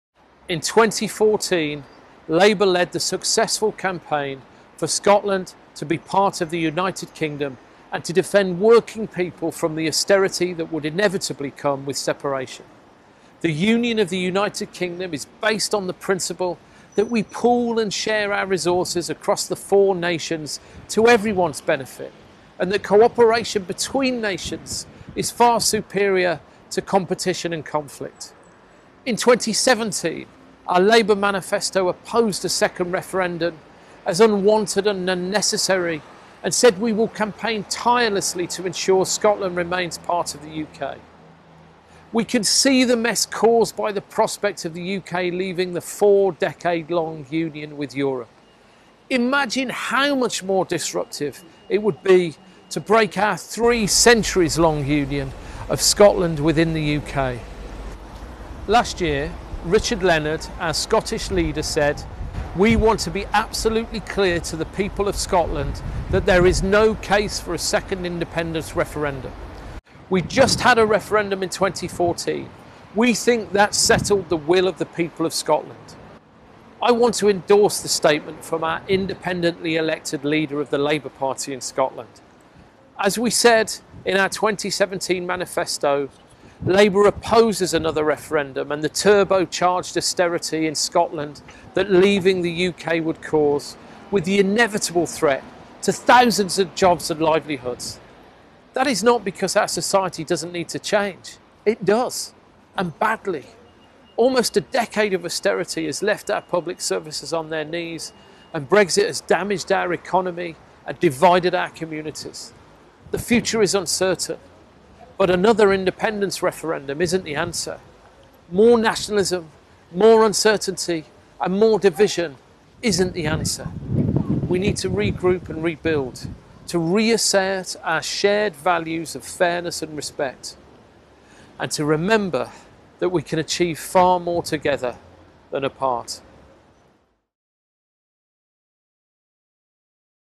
Labour's Tom Watson speaks out against another vote on Scottish independence
The deputy leader said another referendum is not the answer to Tory austerity and Brexit.